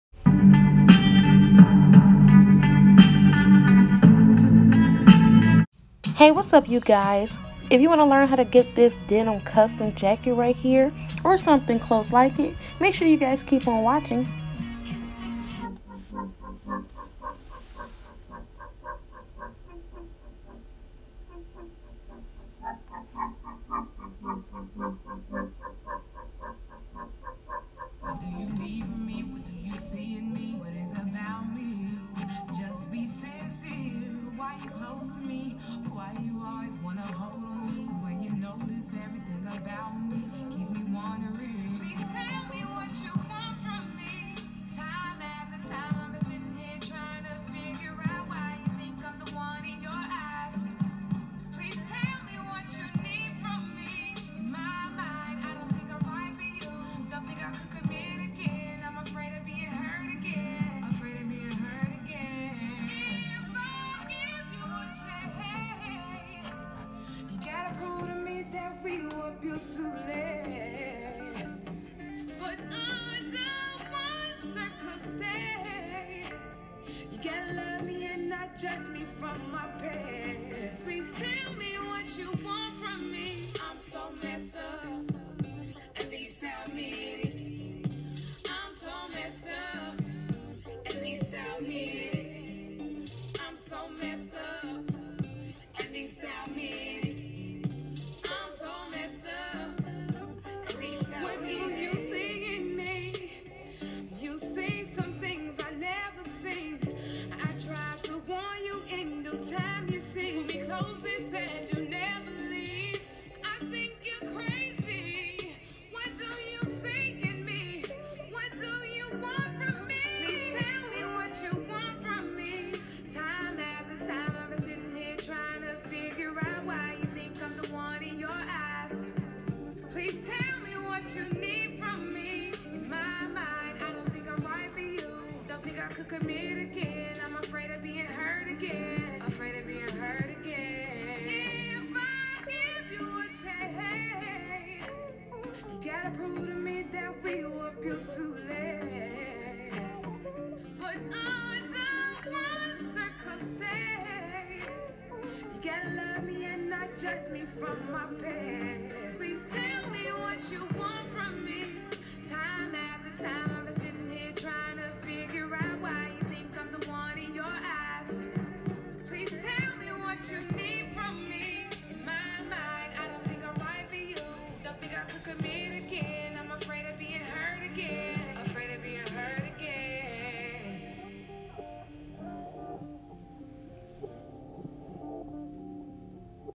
DIY | Custom Denim Jacket
Hey you guys!!! I decided to do another DIY video hopefully you guys enjoy!! This Denim Blue Jean jacket is a must try!! It was sooo cheap who doesn't like that lol 😂 Love you guys!! xoxo 😘

      *Marlesse Greer
      * Ashley Johnson